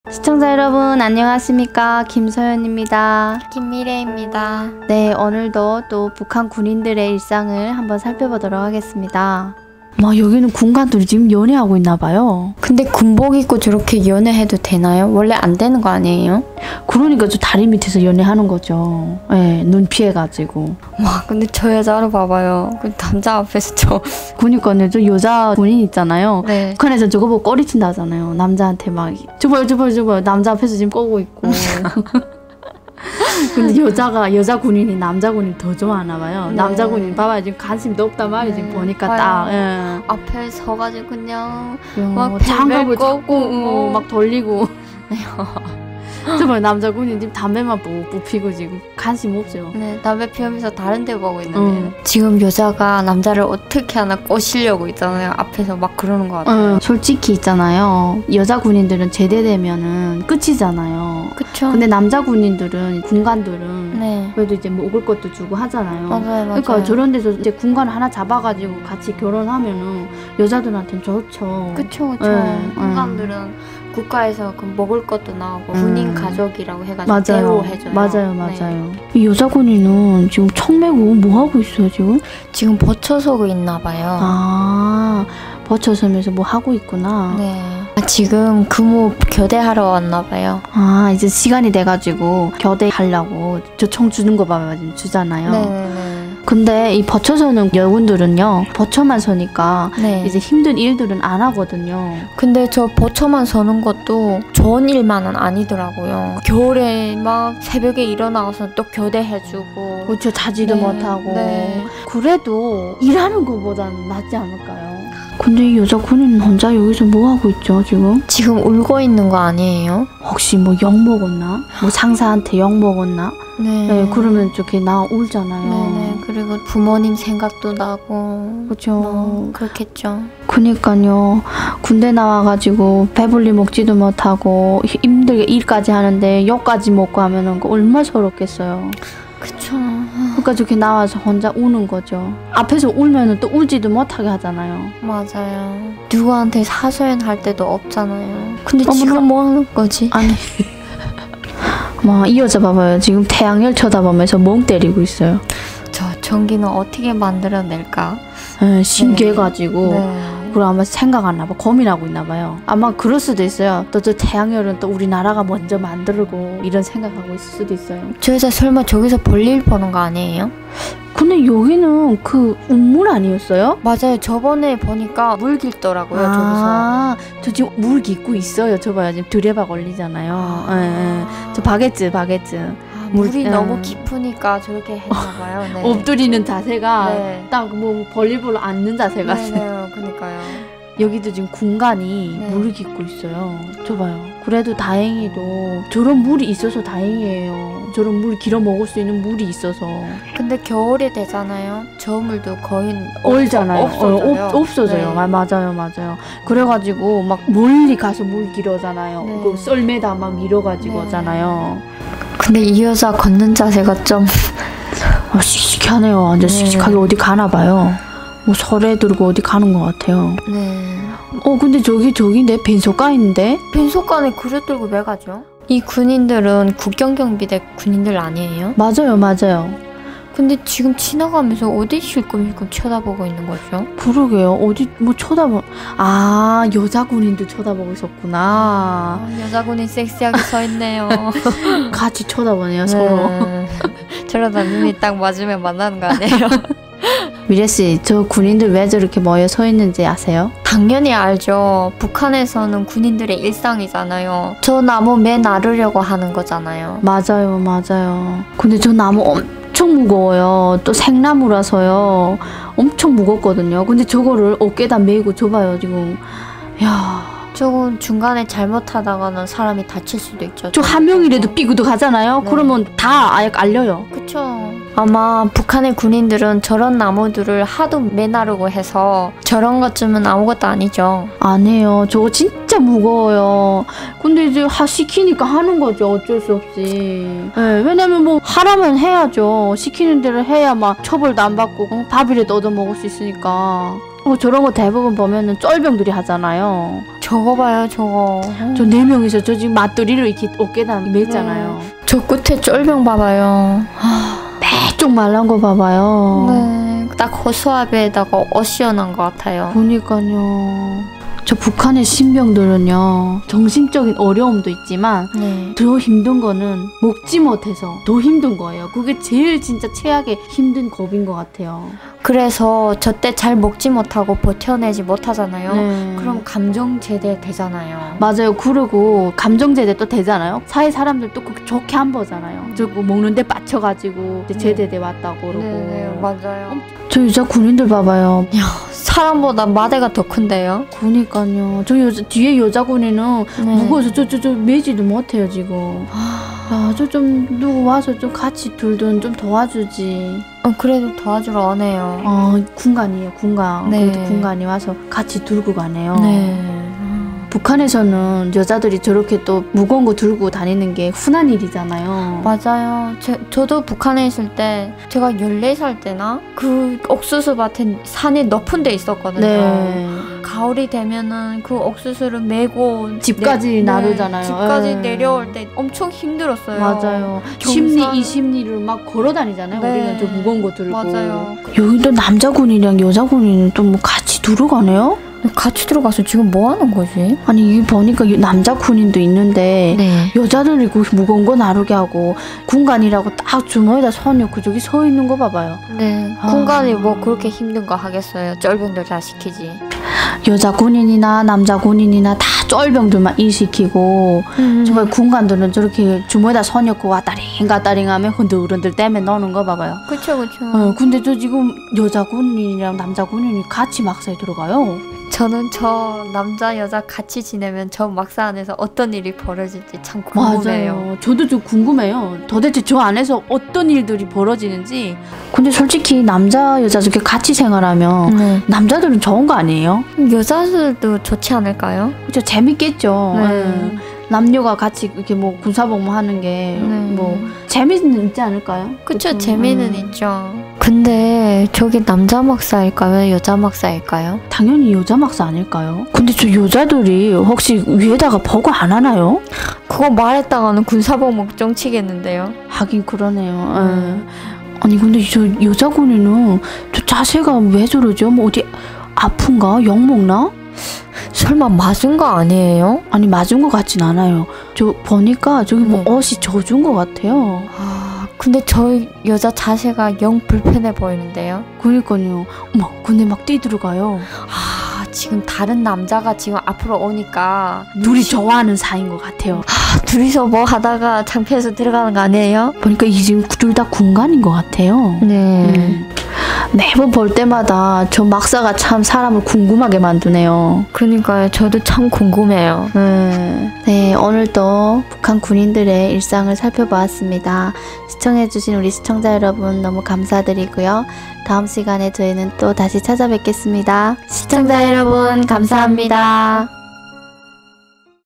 또다시 포착된 북한 남, 여 군인들의 공동 막사생활 그들은 시달림 속에... [오늘의 북한]
우리가 보지 못했던 북한의 또다른 일상 생활을 속속들이 살펴 보는 북한 실제 직캠 영상
더많은 북한일상 실제영상 보기

탈북민과  아리랑